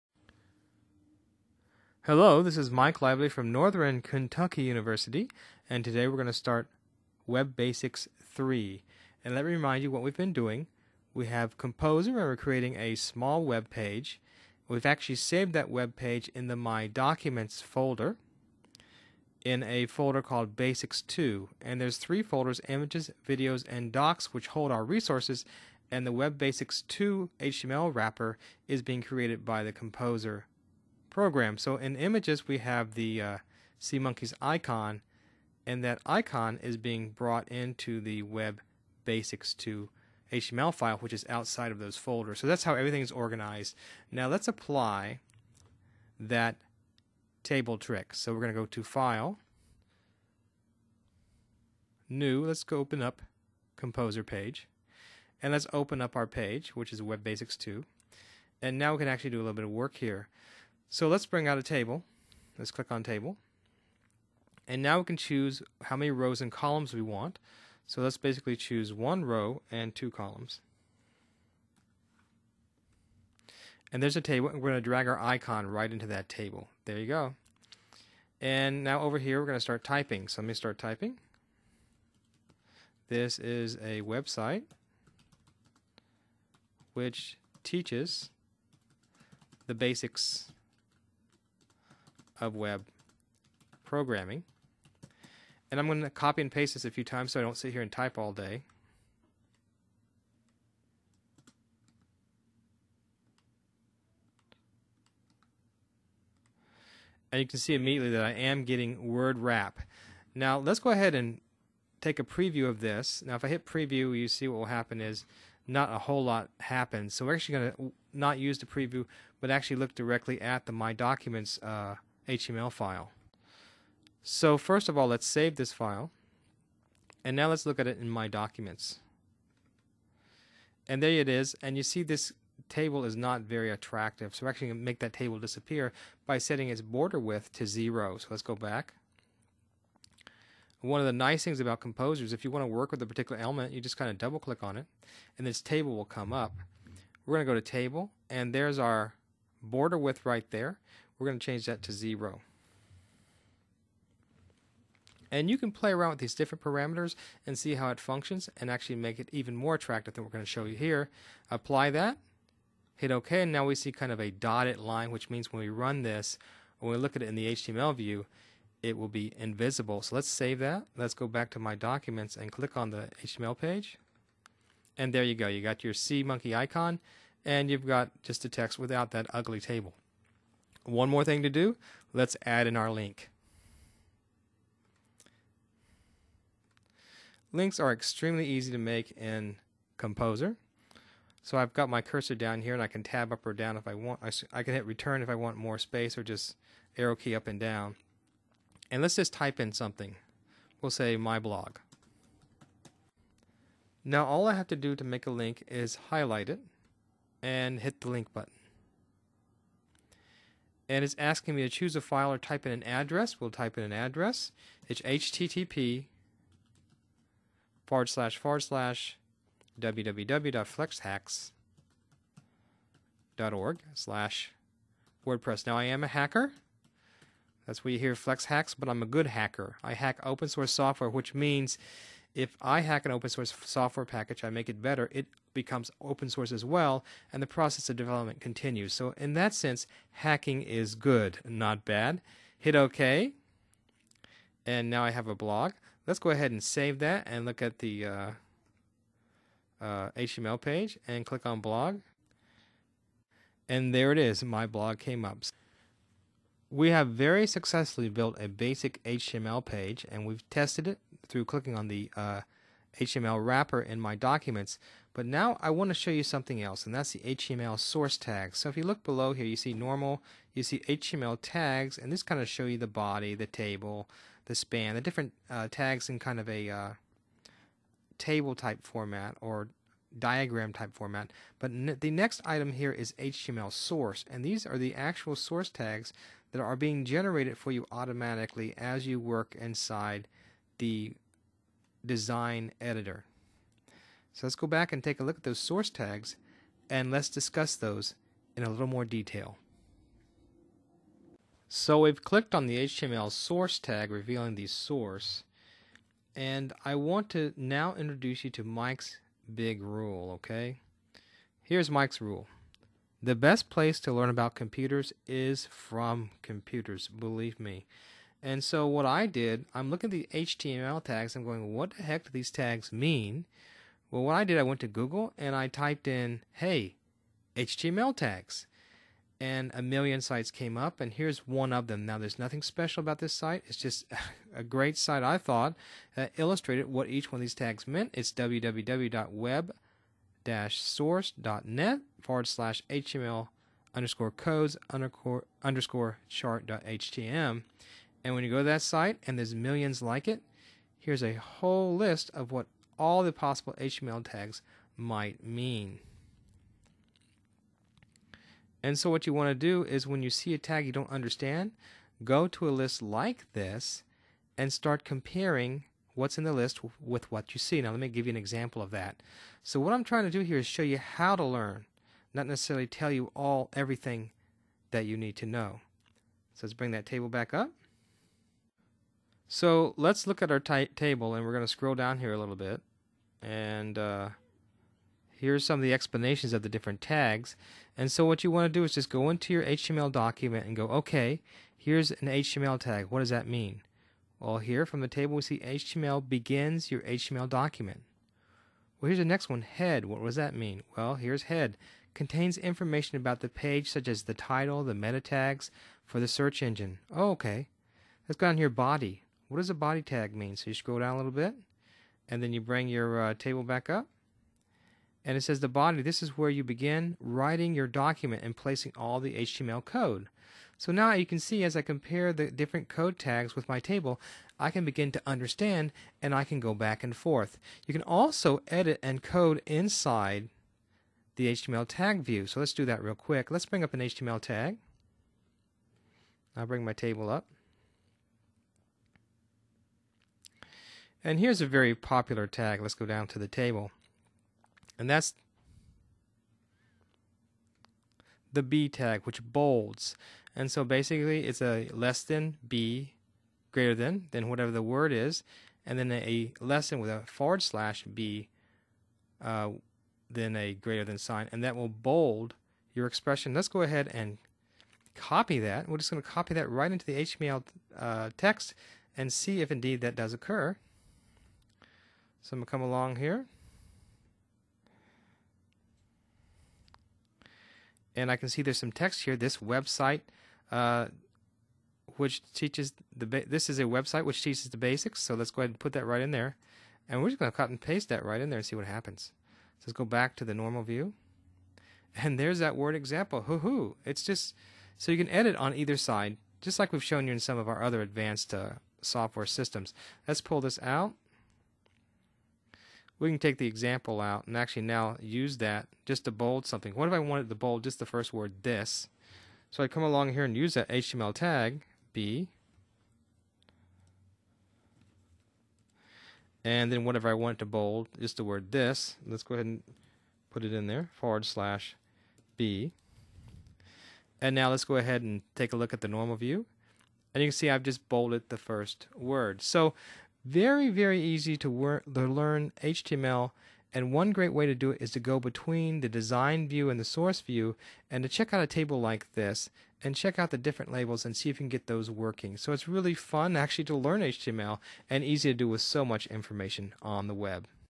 Web Basics Part 3: Using Composer and HTML Tags
In Part 3, we finish our basic HTML page using Composer and demonstrate how to use HTML source tags and give a website for finding out more about these tags.

This tutorial was created by Mike Lively of Northern Kentucky University.